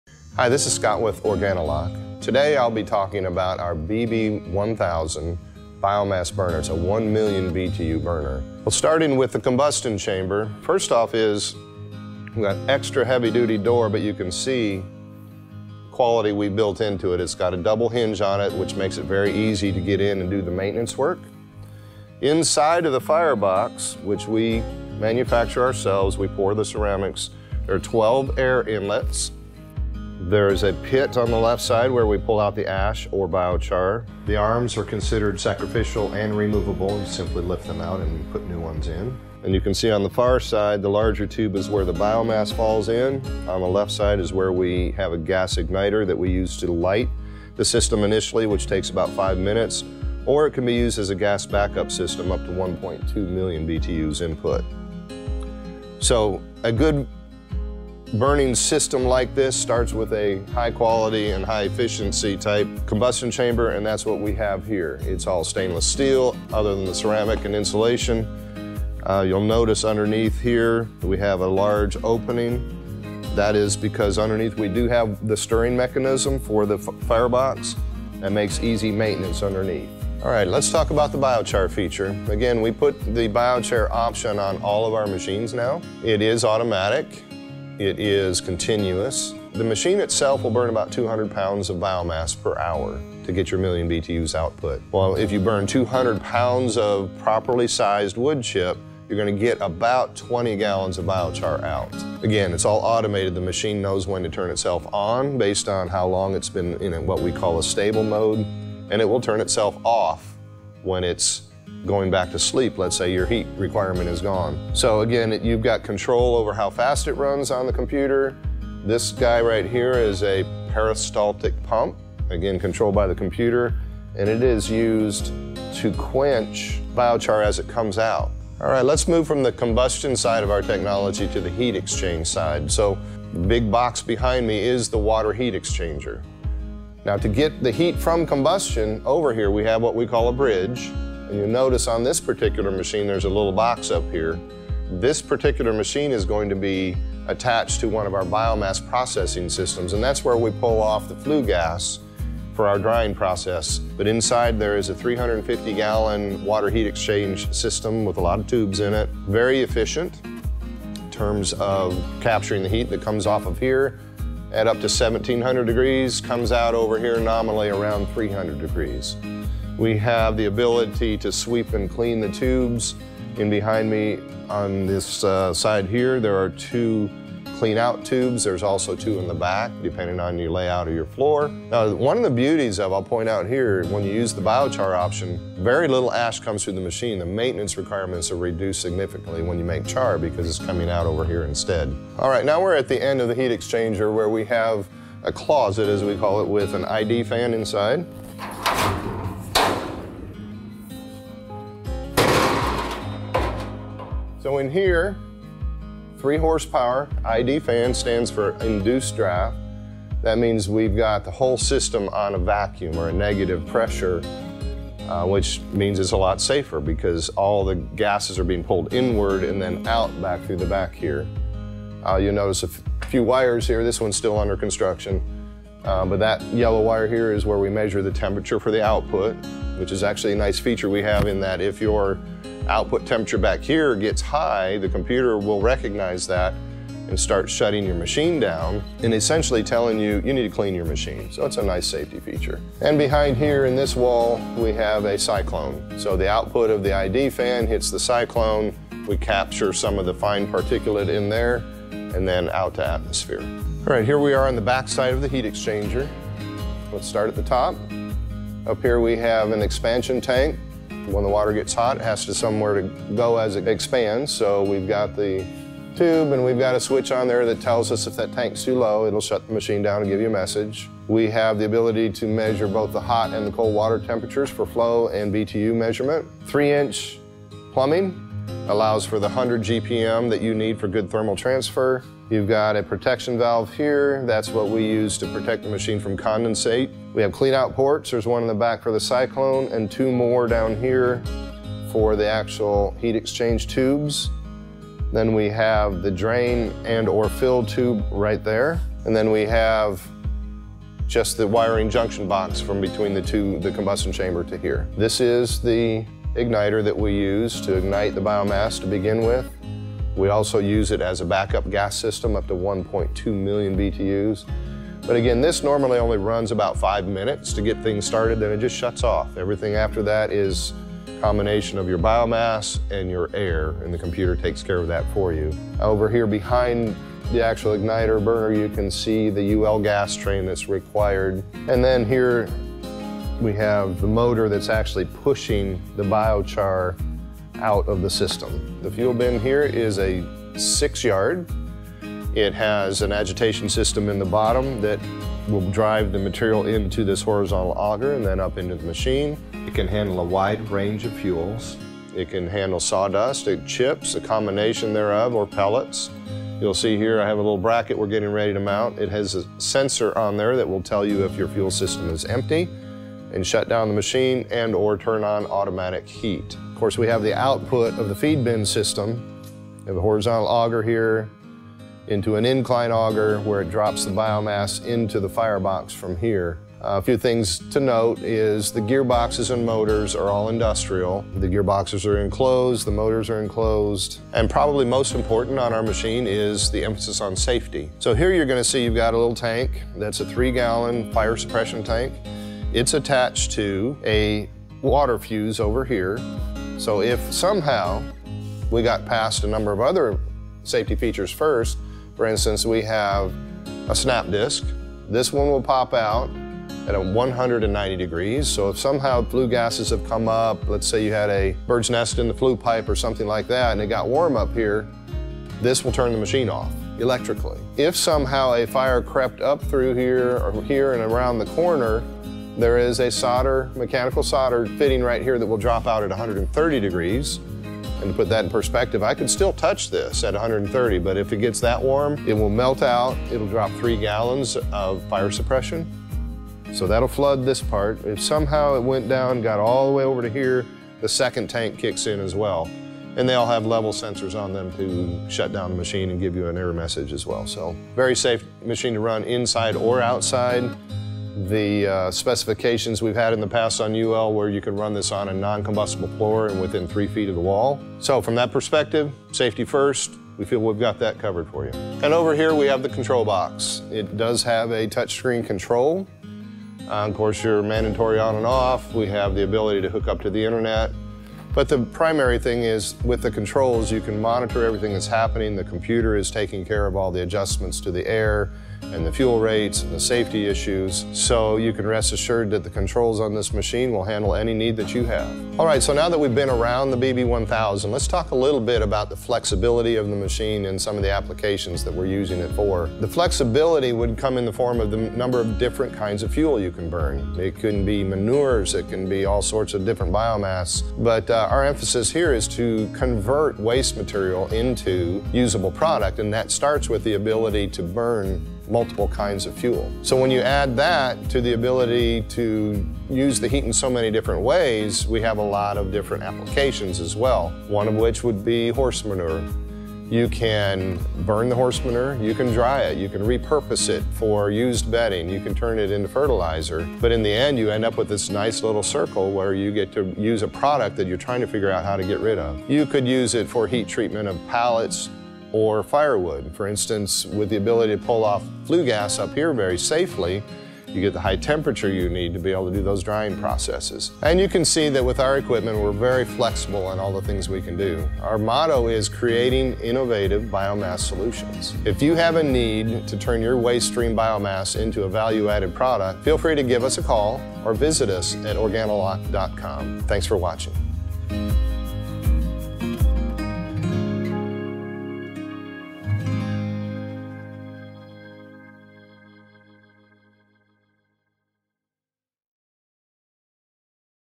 The Bio-Burner model BB-1000 (2019 Revision) by OrganiLock
The Bio-Burner BB-1000 is a multi-fuel biomass heating system and BioChar maker that creates inexpensive heat in the form of hot water.  Woody biomass such as wood chips, sawdust, and wood pellets are the preferred source of fuel.  The BB-1000 is capable of over 1,000,000 BTU output, with automated computer controls, highly efficient and clean burn, and passes all safety standards for indoor and outdoor use.

The BB-1000 can be paired with the BPS-1000 (biomass processing system) as the heat source for processing & drying biomass waste streams.